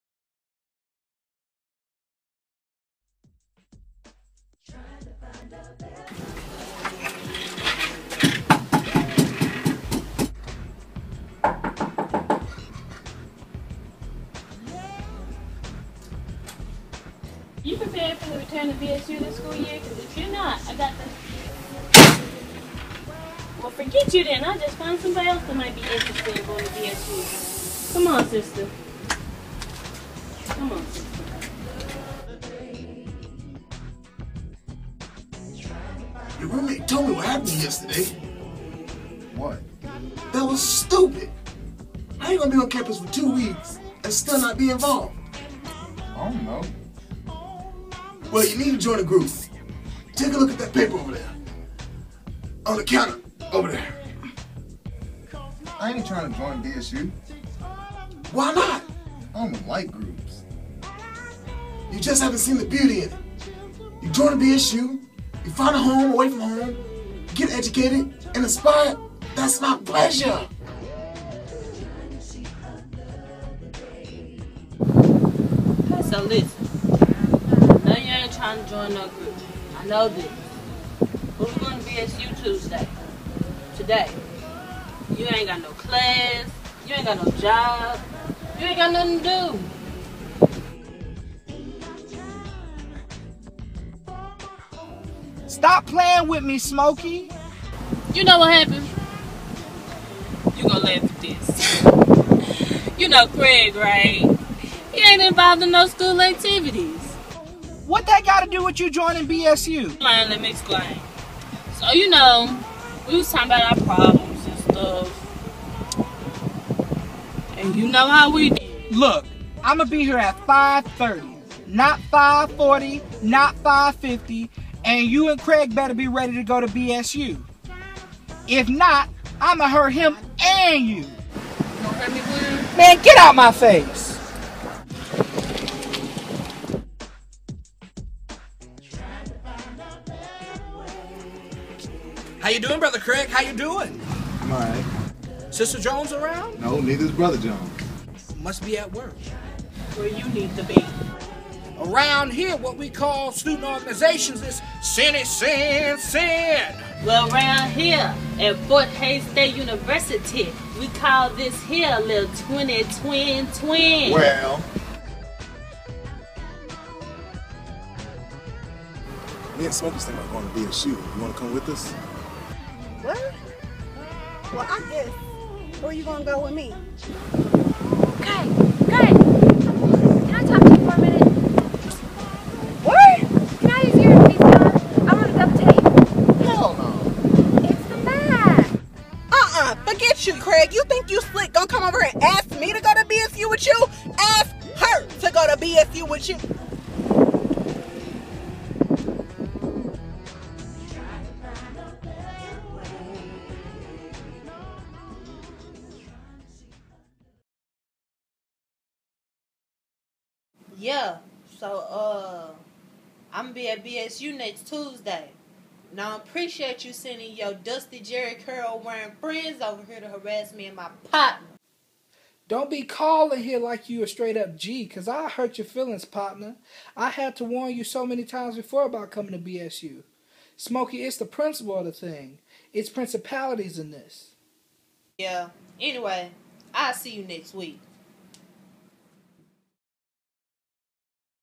Tuesday Movie Trailer
Fort Hays State Universities Black Student Union Presents... Tuesday "Friday Spoof"